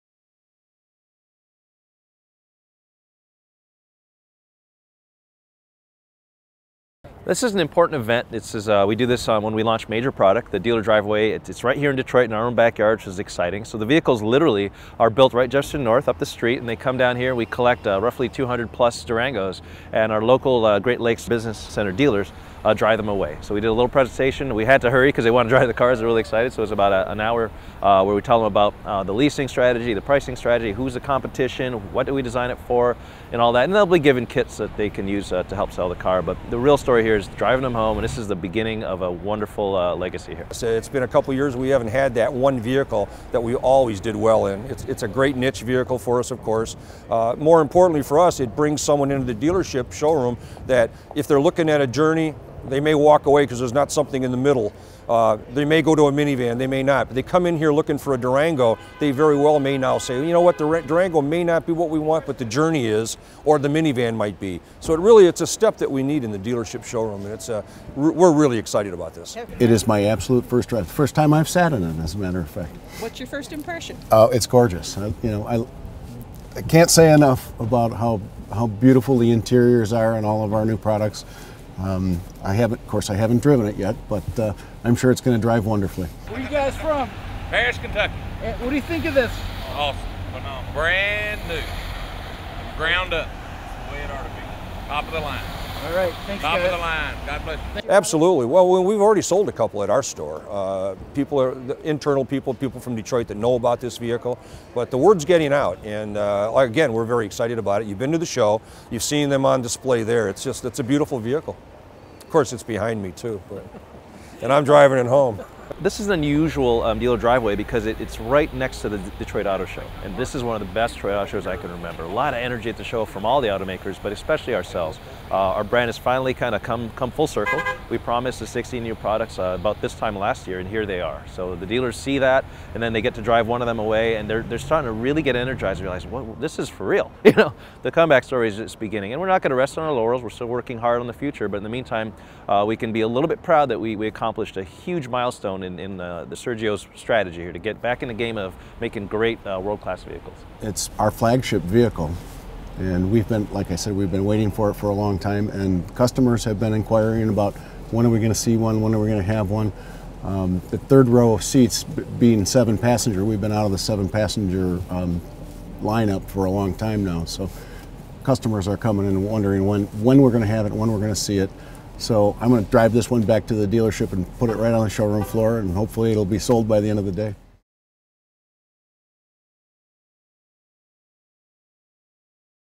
2011 Dodge Durango Drive Away
More then 150 dealerships participated in the 2011 Dodge Durango Drive Away. Dodge Brand President & CEO Ralph Gilles plus a few dealers talk about the event's importance and the new Durango.